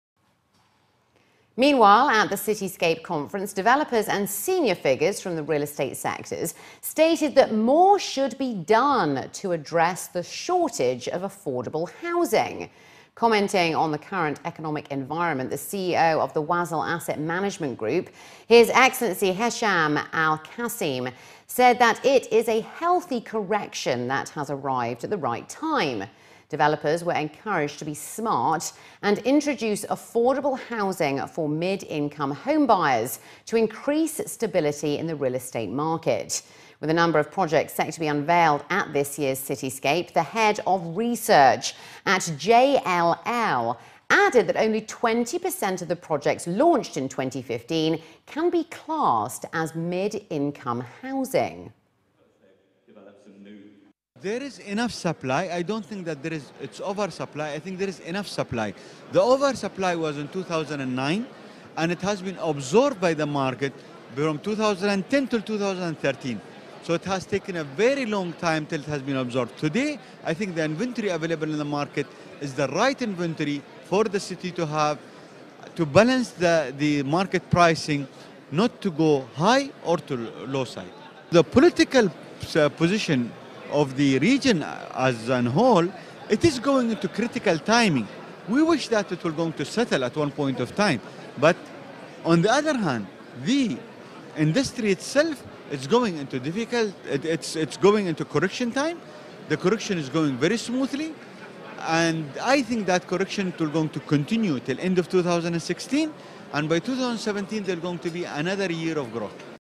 Hesham Al Qassim's interview by City 7 TV at Cityscape Global conference
wasl Asset Management Group's CEO, Hesham Al Qassim, interviewed by City 7 TV at Cityscape Global conference - Dubai 2015